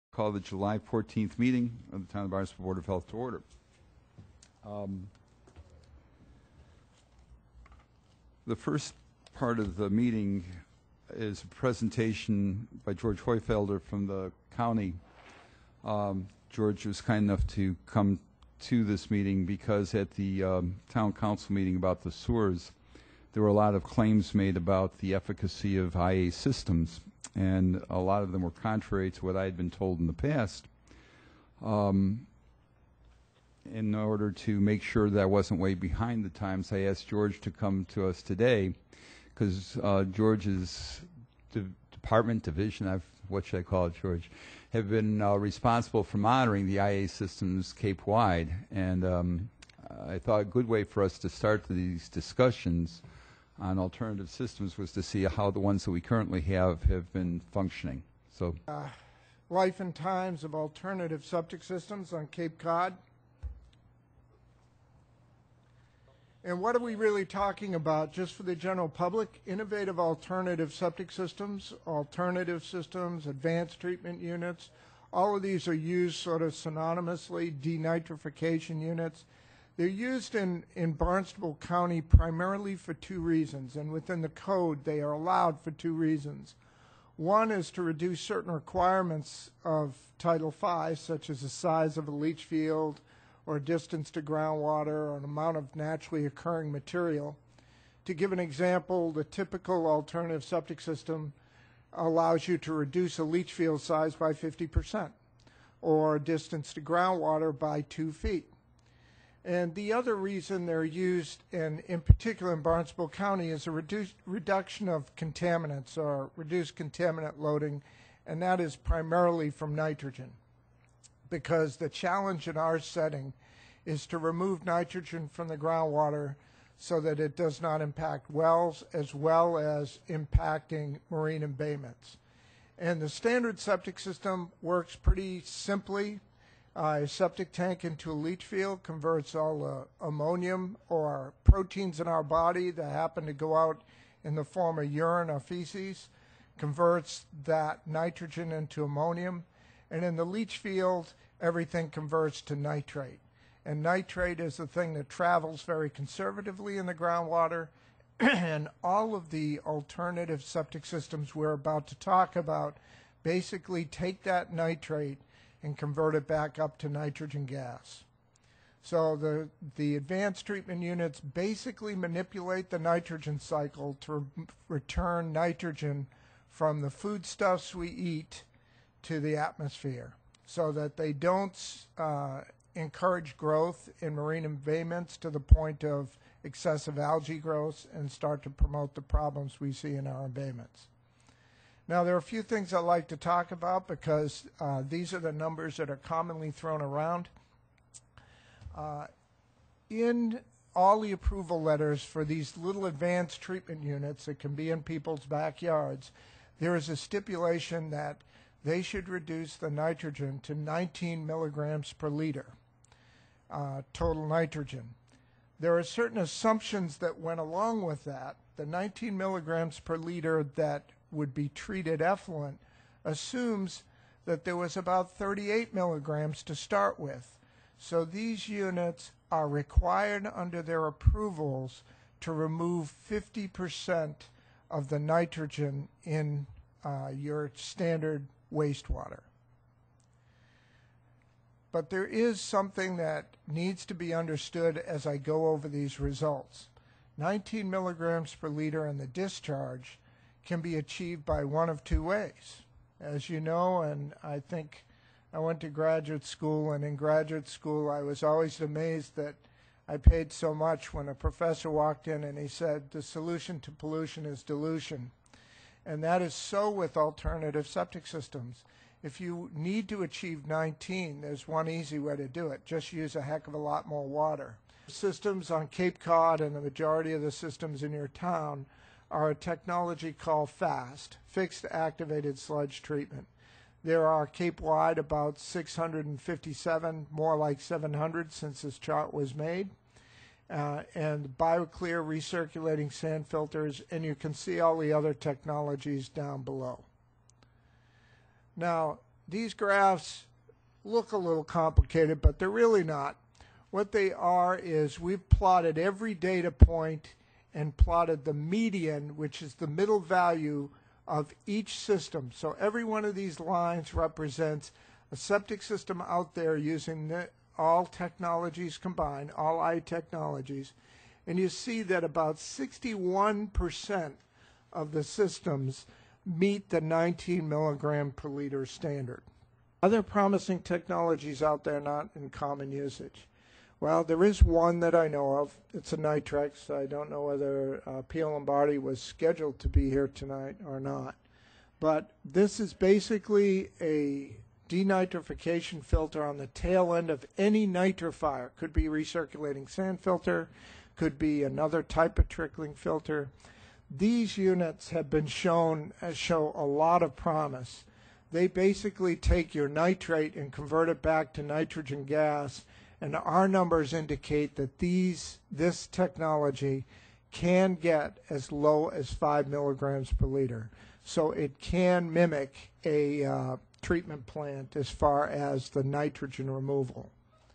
The two solutions to groundwater pollution are dilution and Nitrex...
Barnstable County Health Director, George Huefelter, tells Barnstable Board of Health about alternative septic systems